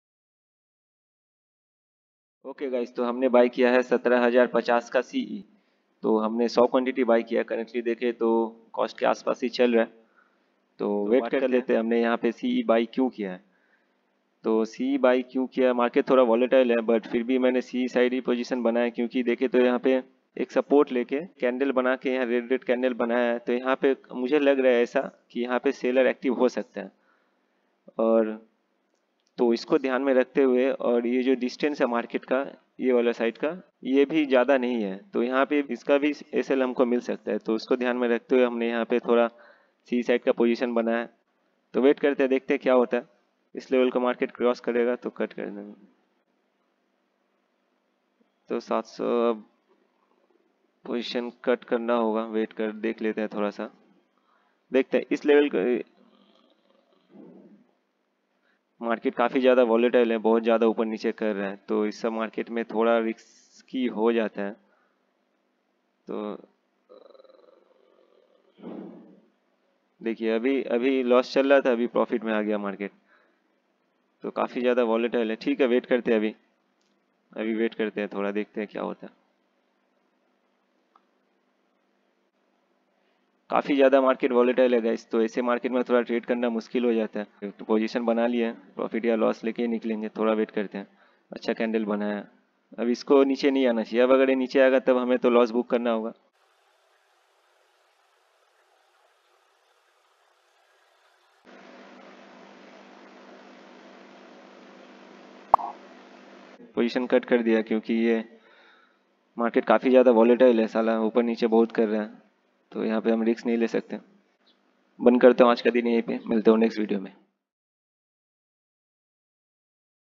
🔴Live trading video // 13 October 2022//Sl Hunter //learning with mistake// Being Trader
On Being Trader YouTube Channel you will learn Price
Action Trading in stock market. In this video We have
shown my live intraday trading in bank nifty option buying
using pure price action strategies. No need to rely on
fancy buy/sell signal giving indicators. Indicators are just
like make money online like schemes. Just Observe and
to get all the latest updates!
Instagram : I'm on Instagram as being_trader_kolkata26. Install the app to follow my photos and videos.